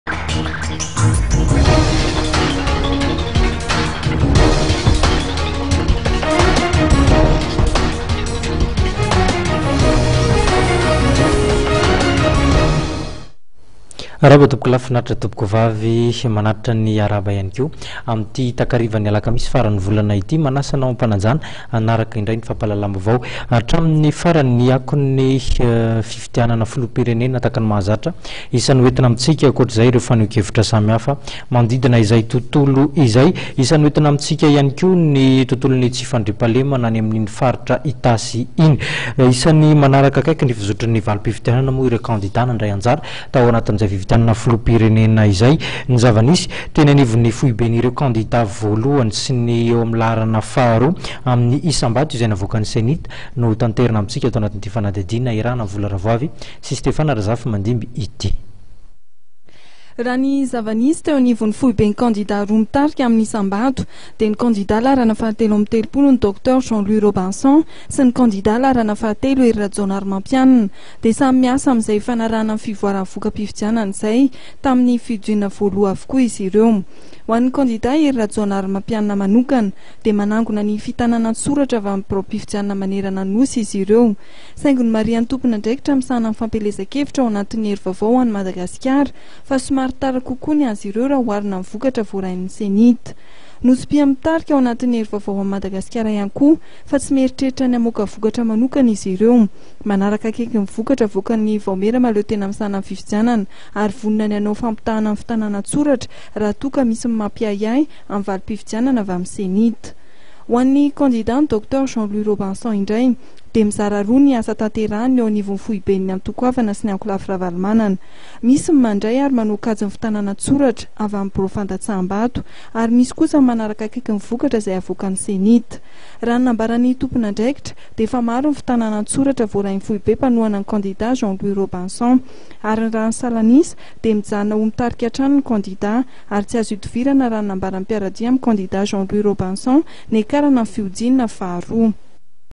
01 AMBIANCE QG
Manaraka akaiky ny fizotry ny valim-pifidianana ny  kandida ho filoham-pirenena. Ny zava-misy eo anivon'ny foiben'ny kandida voalohany sy faharoa amin'ny isam-bato navoakan'ny CENIT no entina amin'ity fanadihadihana manaraka ity.